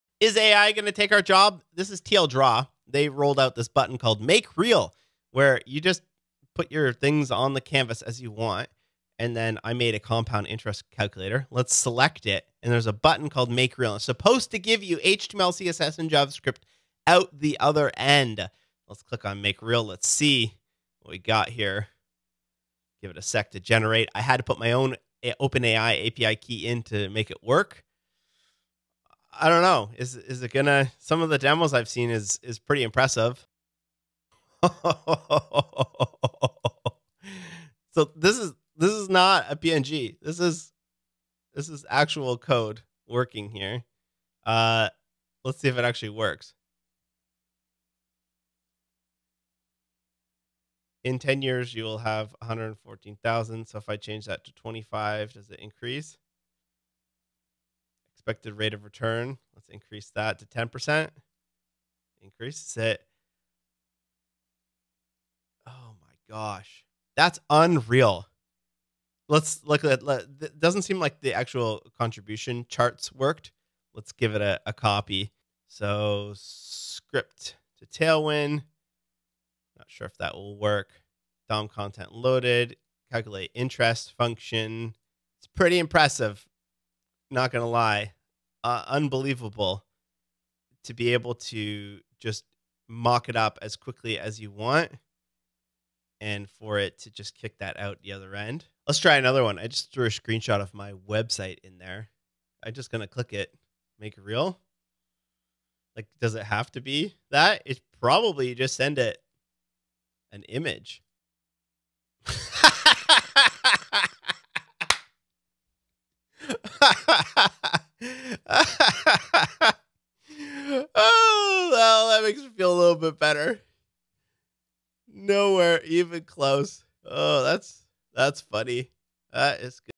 Unreal. AI writing Tailwind + JS from a sketch
Trying the new TLDraw "Make Real" Button